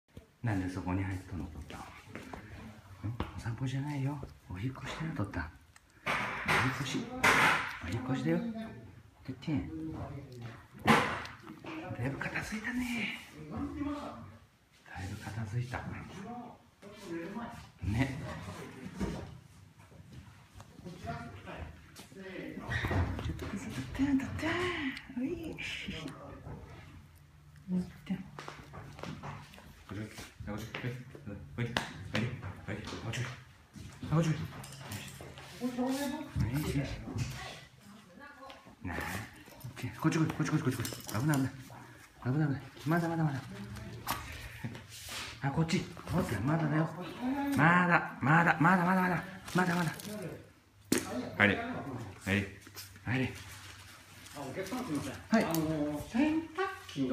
長年住んだマンションから、いよいよ新居へチワワと引越しです！
チワワのチョッピさん、間もなく出発ですが散歩に行くんだと勘違いしていますｗ